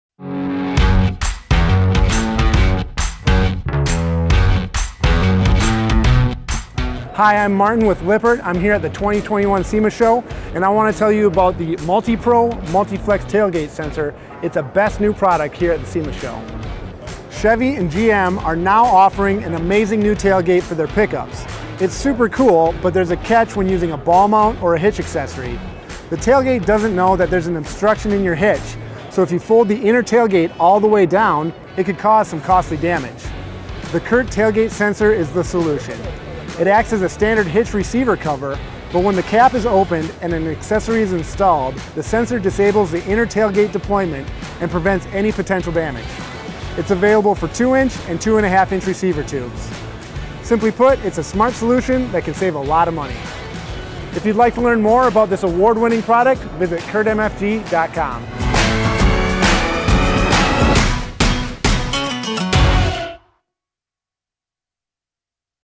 SEMA 2021 - Award Wining New Product - CURT Multipro/Multiflex Tailgate Sensor
Martin tells us a little bit about this award-winning new product.  When towing with your GMC Sierra or Chevy Silverado, it is important to protect your tailgate. A ball mount or other hitch accessory inserted in the receiver can become an obstruction for the inner tailgate operation. This hitch cap sensor is a tailgate saver, working in sync with the truck’s electronics to prevent the tailgate from folding down and being damaged by the hitch accessory.

Installing this Chevrolet / GMC tailgate sensor is simple with no wire cutting or splicing required. It fits virtually any 2" x 2" receiver hitch, and it requires no extra electrical components for a complete installation.